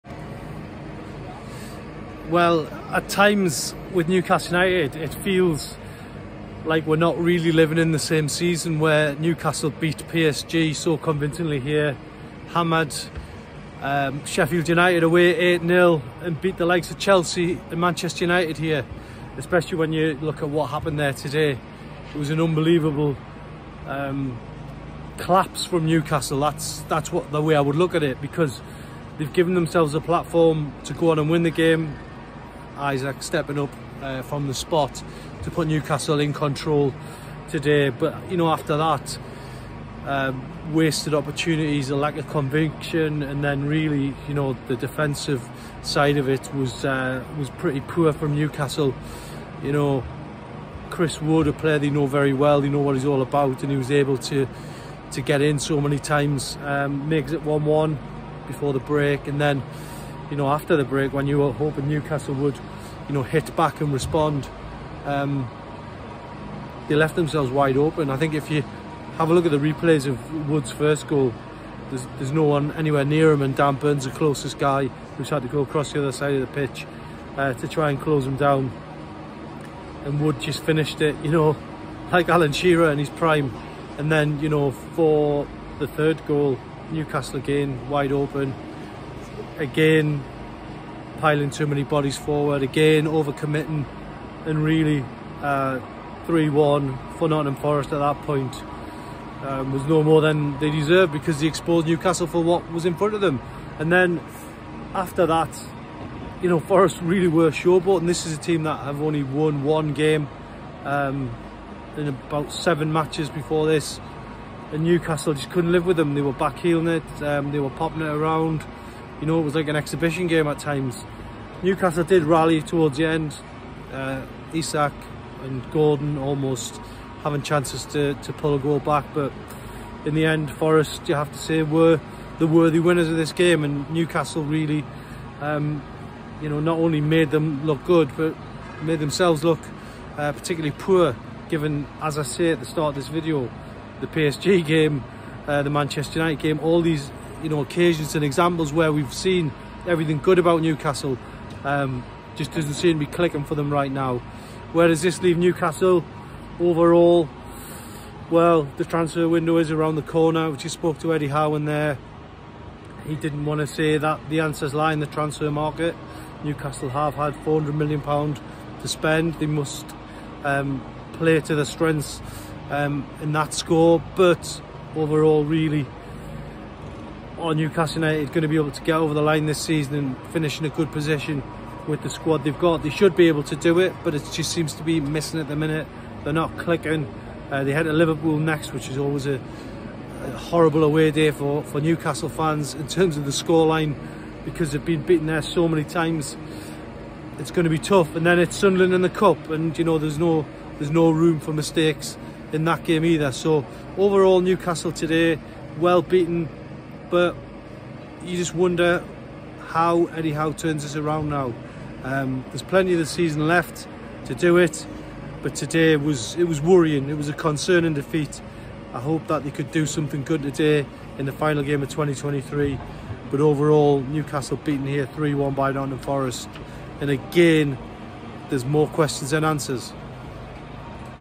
Like living in a DIFFERENT season - my gut reaction after Newcastle United beaten by Forest
Newcastle United were poor in the second half against Nottingham Forest